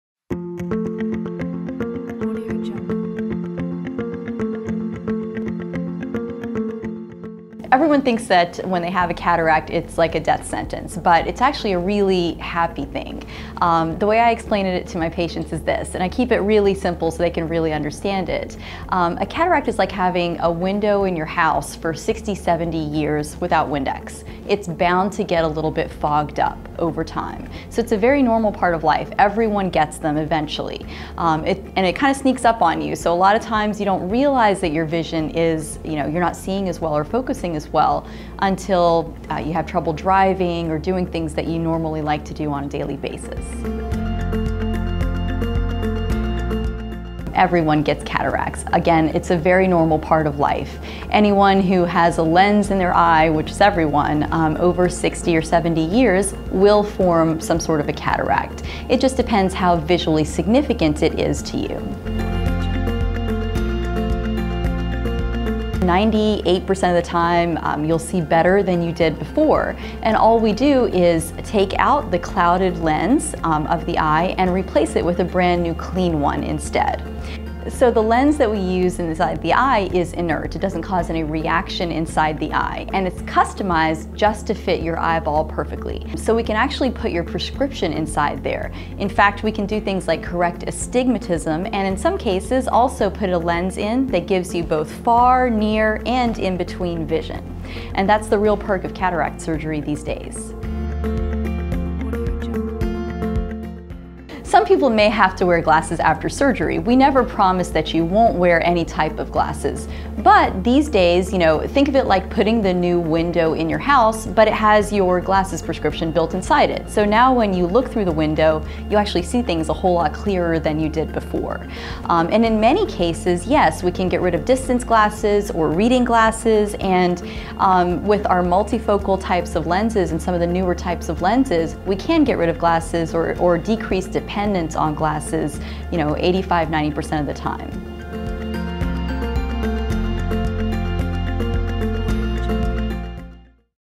Ranya G. Habash, M.D. discusses cataract surgery
Dr. Ranya G. Habash, Voluntary Assistant Professor of Clinical Ophthalmology at Bascom Palmer Eye Institute at the University of Miami Miller School of Medicine discusses cataract surgery
-What is a cataract?
-Who gets cataracts?
-What are the advantages of cataract surgery?
-Do I have to wear glasses after surgery?

Facebook: Bascom Palmer Eye Institute